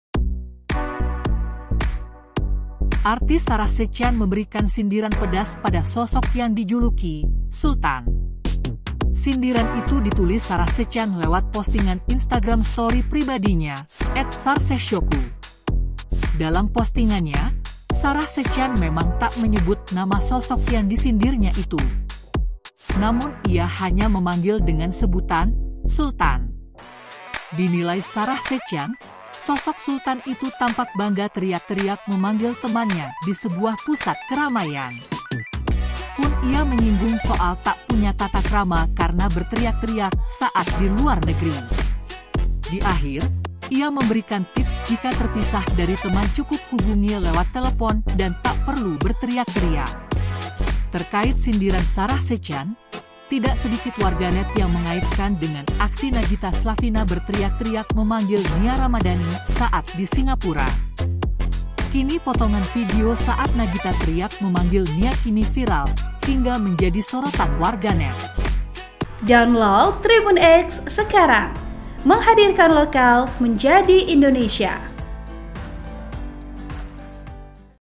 Sarah Sechan Sindir Sosok Sultan Tak Punya Tata Krama
TRIBUNNEWS.COM - Artis Sarah Sechan memberikan sindiran pedas pada sosok yang dijuluki 'sultan'.

Sindiran itu ditulis Sarah Sechan lewat postingan Instagram Story pribadinya, @sarsehshoku.

Dalam postingannya, Sarah Sechan memang tak menyebut nama sosok yang disindirnya itu.

Namun ia hanya memanggil dengan sebutan 'sultan'.

Dinilai Sarah Sechan, sosok sultan itu tampak bangga teriak-teriak memanggil temannya di sebuah pusat keramaian.

Pun ia menyinggung soal tak punya tata krama karena berteriak-teriak saat di luar negeri.

Di akhir, ia memberikan tips jika terpisah dari teman cukup hubungi lewat telepon dan tak perlu berteriak-teriak.

Terkait sindiran Sarah Sechan, tidak sedikit warganet yang mengaitkan dengan aksi Nagita Slavina berteriak-teriak memanggil Nia Ramadhani saat di Singapura.

Kini potongan video saat Nagita teriak memanggil Nia kini viral hingga menjadi sorotan warganet.